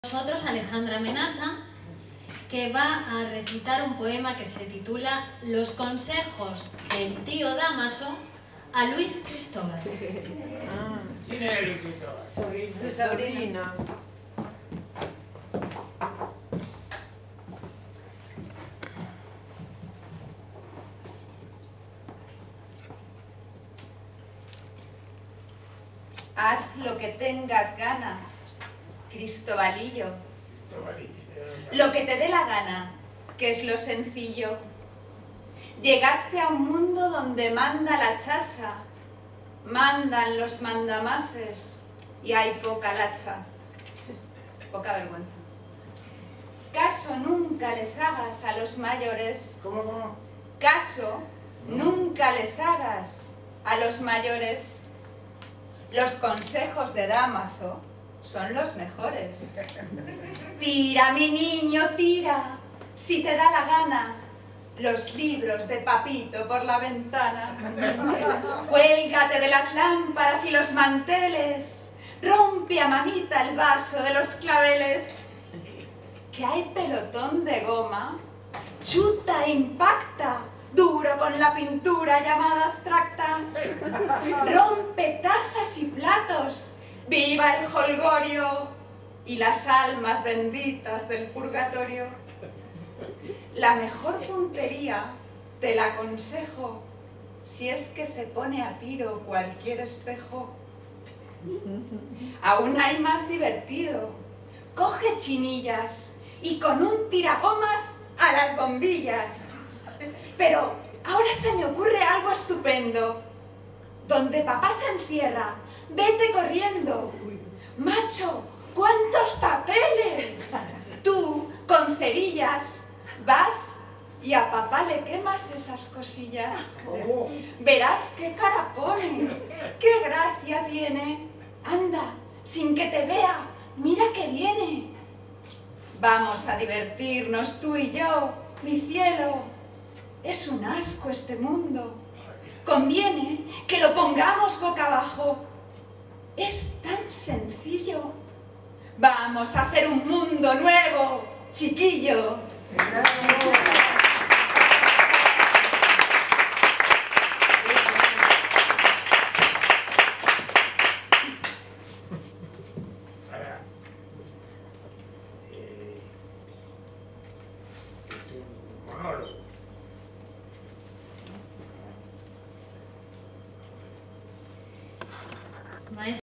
ALEJANDRA MENASSA LEE LOS CONSEJOS DEL TÍO DAMASO A LUIS CRISTOBAL
Recital de poesía de DAMASO ALONSO en la sede de Grupo Cero
Calle Princessa 12, 1izq - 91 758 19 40
ESCUELA DE POESÍA GRUPO CERO